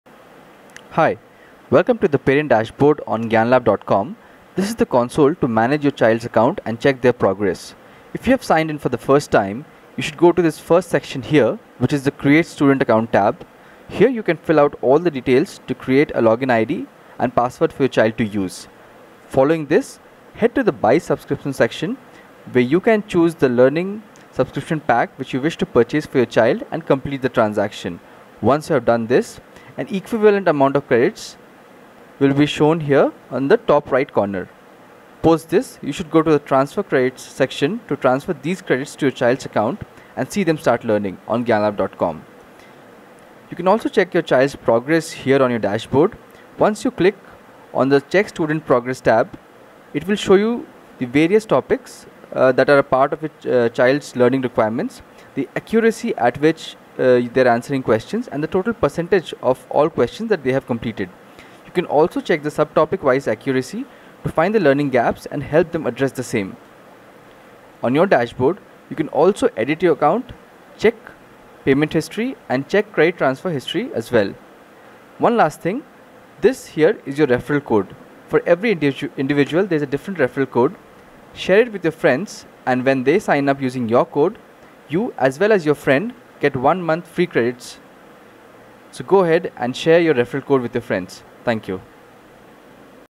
How to use GyanLab com - Parent Dashboard
This video shows you how to optimally use the Parent Dashboard on GyanLab.com to help your kids start learning on our awesome learning platform.